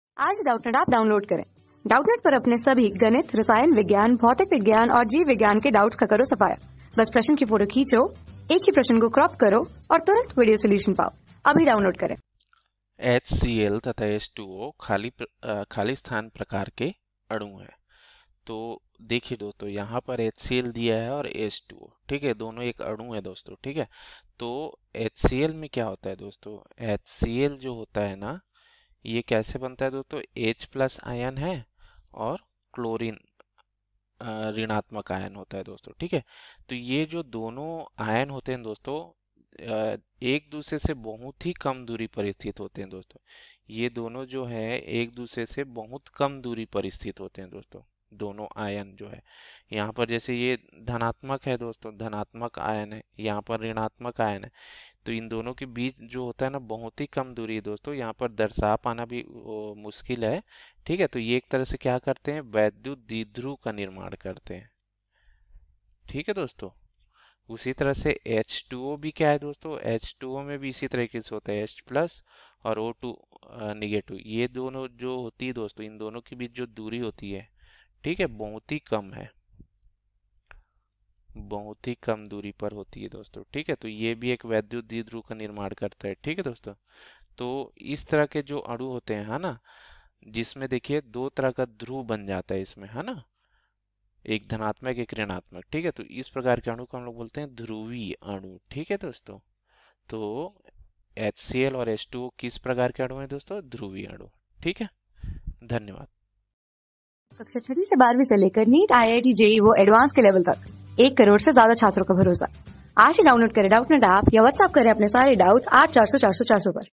HCl तथा H_(2)O ......... प्रकार के अणु हैं। | 12 | संधारित्र तथा स्थिर-विद्युत जनित्र | PHYSICS...
HCl तथा H_(2)O प्रकार के अणु हैं।
Class: 12
Subject: PHYSICS
Chapter: संधारित्र तथा स्थिर-विद्युत जनित्र
Board:Bihar Board

👉 Have Any Query? Ask Us.